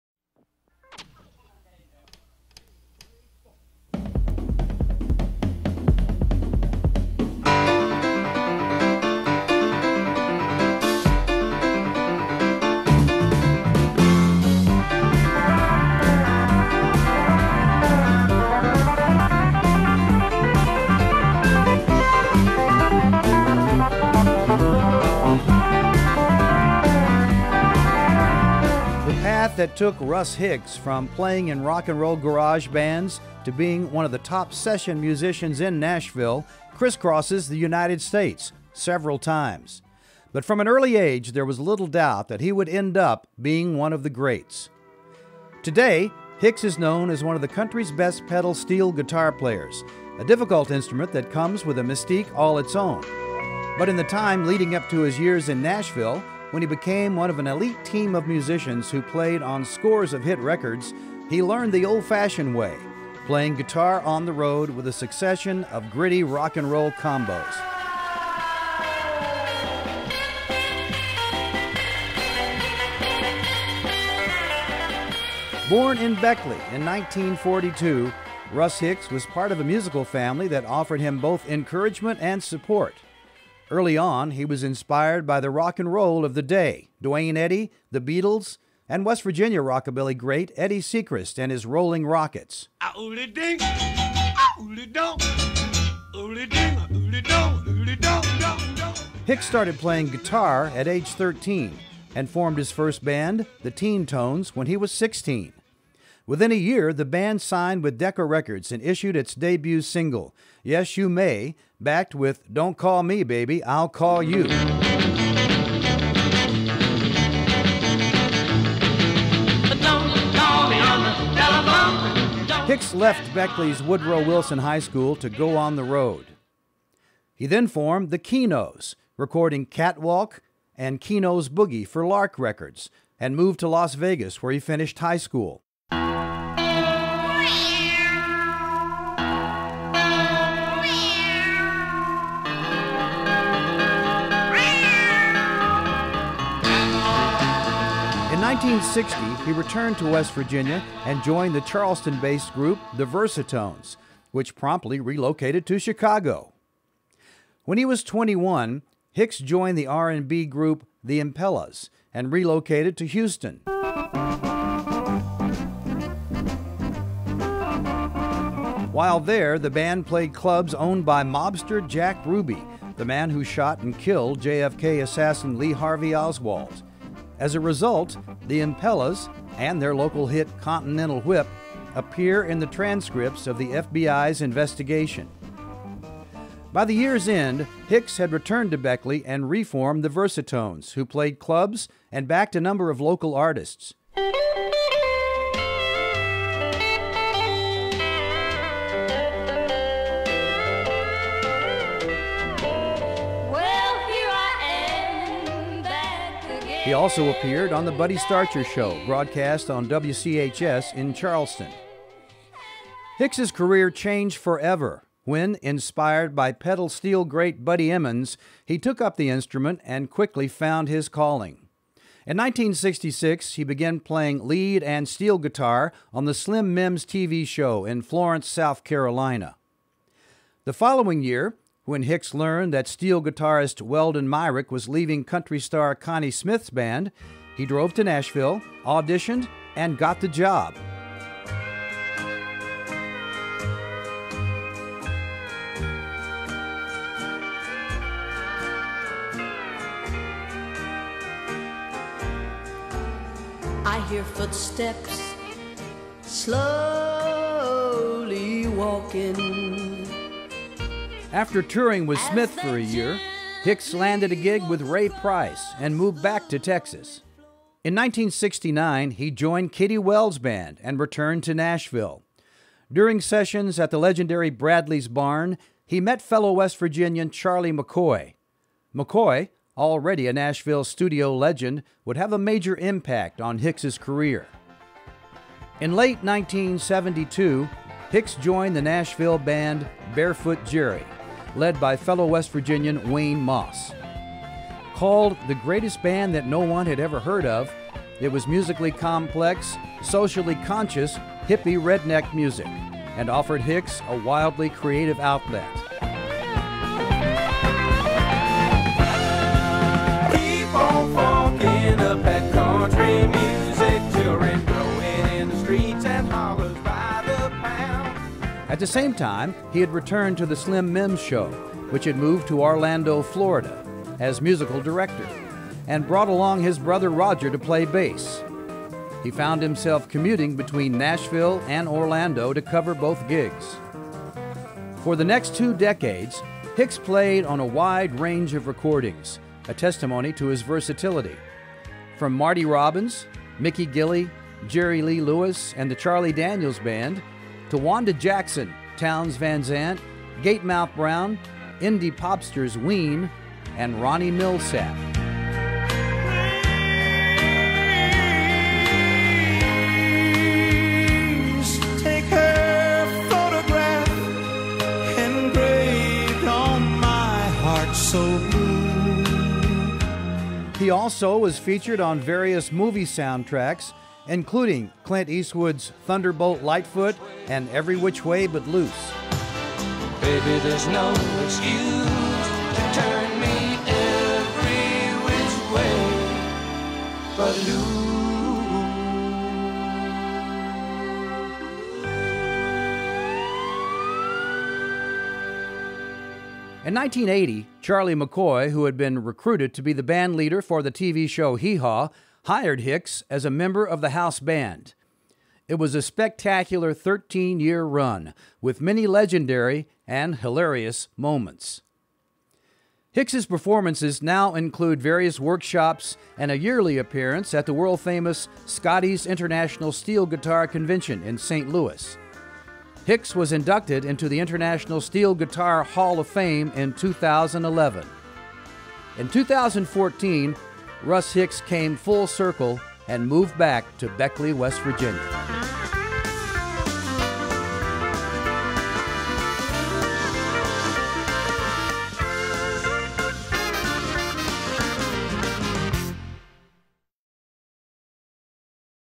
Russ Hicks 2015 West Virginia Music Hall Of Fame Induction Vignette
2015 West Virginia Hall Of Fame Induction Vignette
Produced by Brainwrap Productions
Script by Michael Lipton and Gavin Wissen
Narration by Larry Groce
Engineering by Francis Fisher
Additional Research by Gavin Wissen